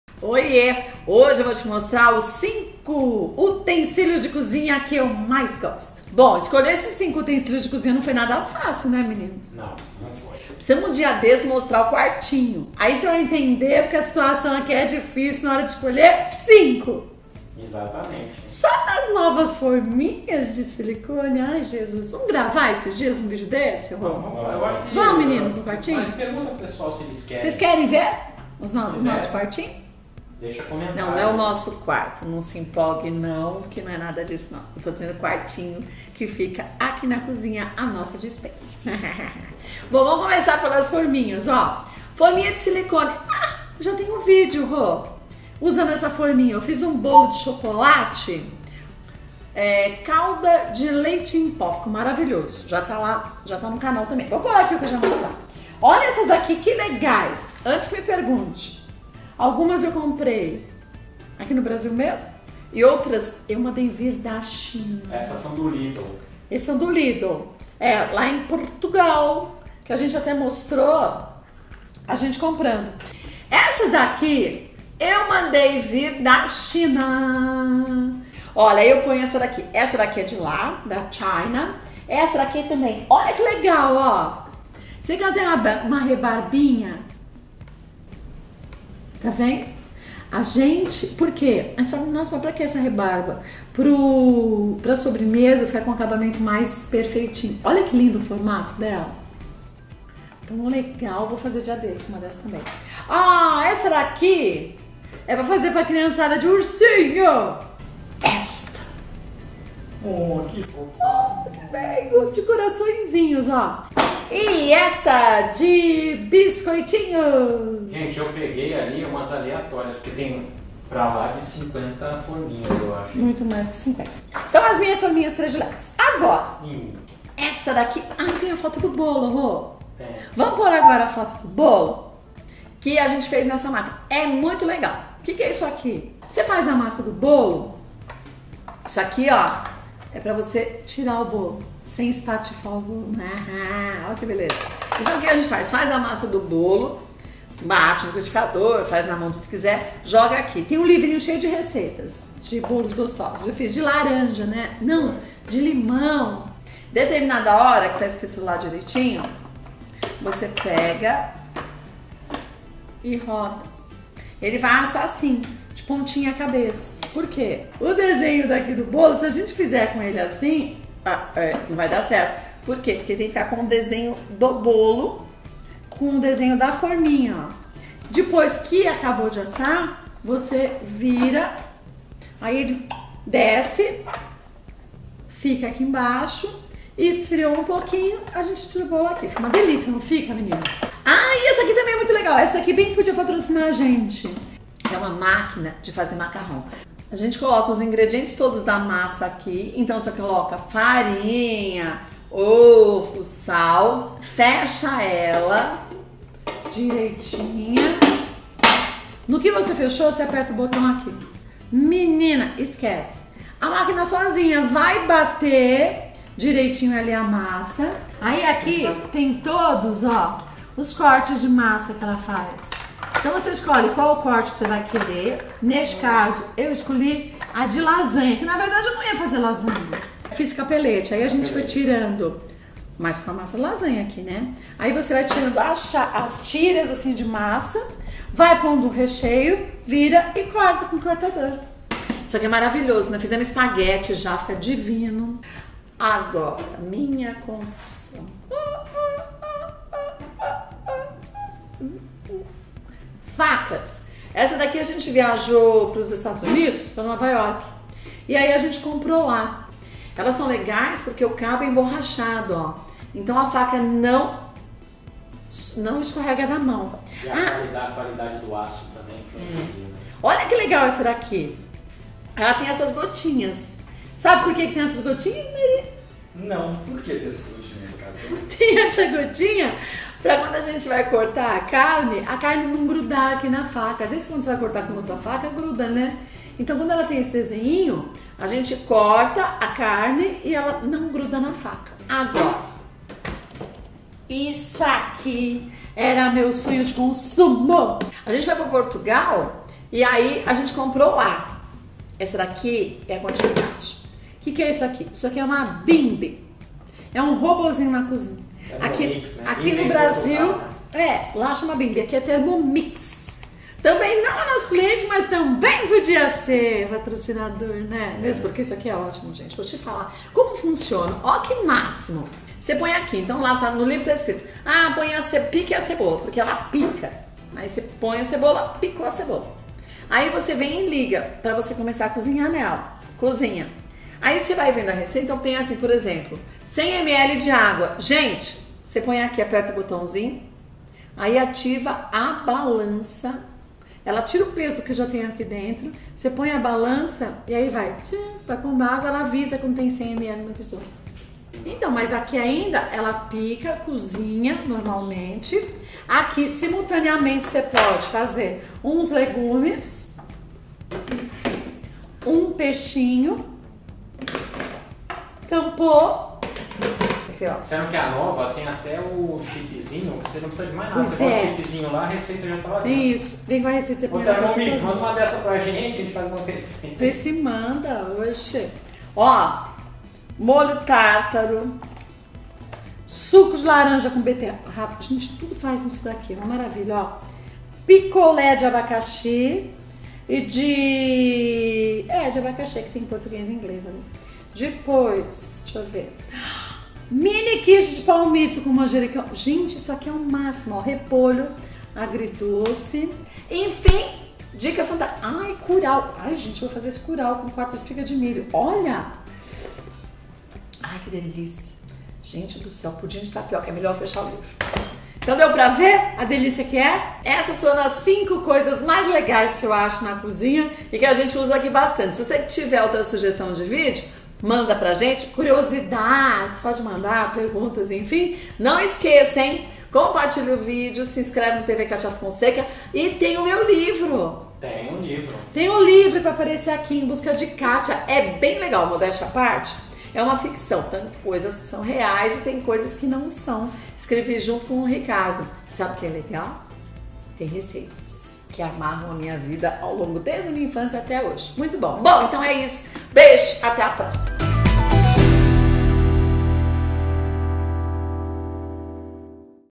5 Coisa Que Amo na Minha Cozinha - TAG
Neste vídeo você vai descobrir quais as 5 coisas que a Catia Fonseca mais ama na sua cozinha.

App Baixe o APP do TV Catia Fonseca é só clicar no link abaixo: